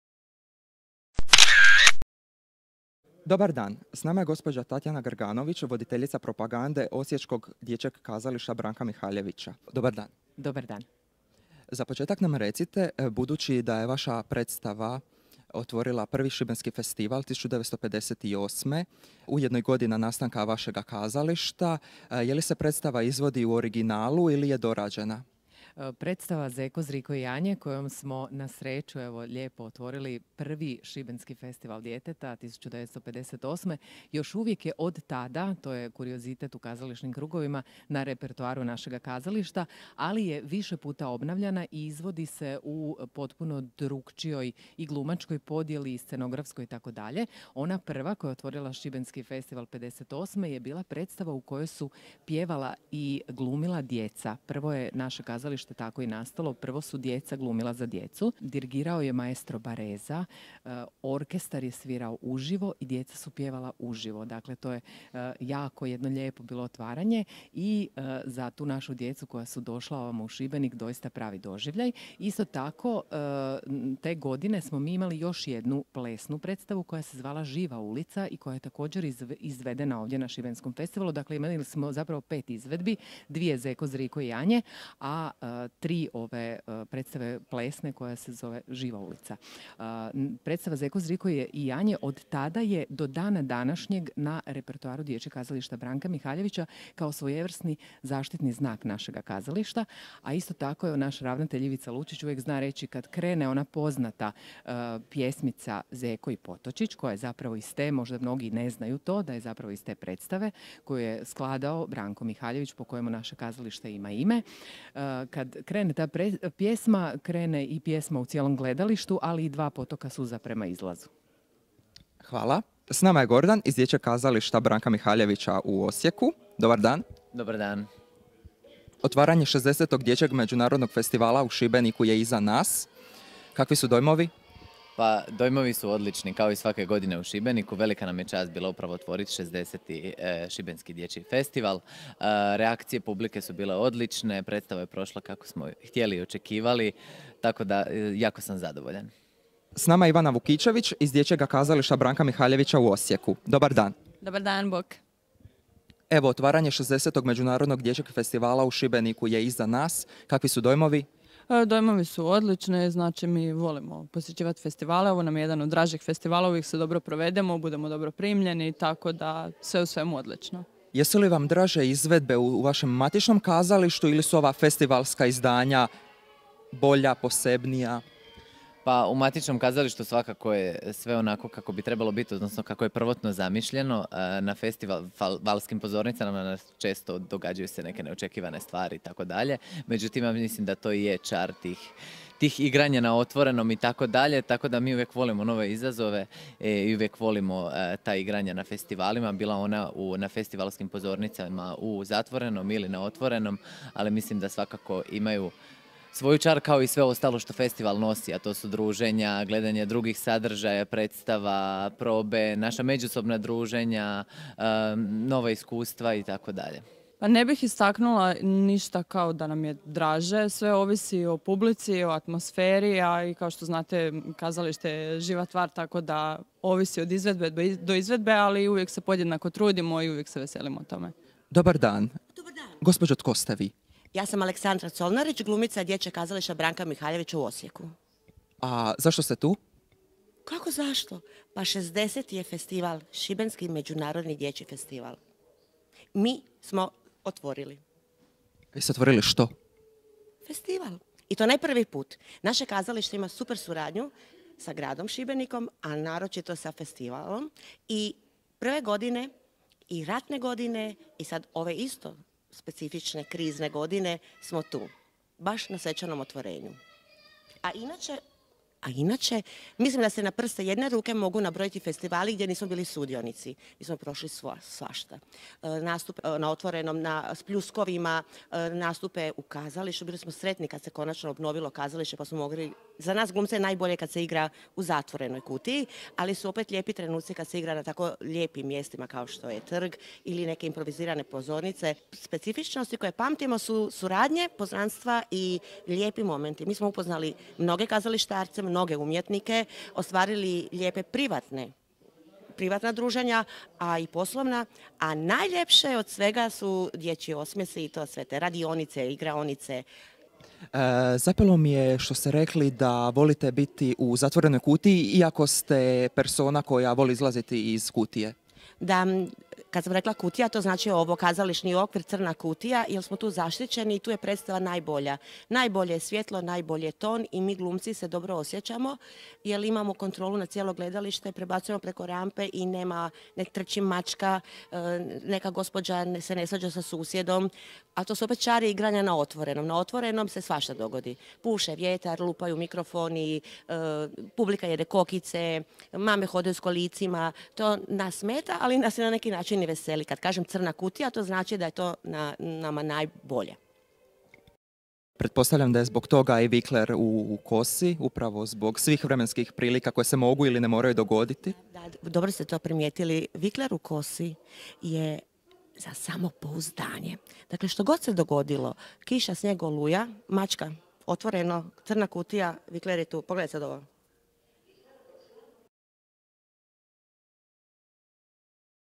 DK Branka Mihaljevića u Osijeku | MDF
Glumci dječjeg kazališta Branka Mihaljevića otvorili su 60. Festival predstavom "Uspavanka za mišića", a davne 1958. otvorili su 1. MDF u Šibeniku s predstavom Zeko, zriko i janje. U intervjuu ćete čuti Tatjanu Grganović, voditeljicu propagande, akademske glumce Gordana Marijanovića i Ivanu Vukićević te Aleksandru Colnarić.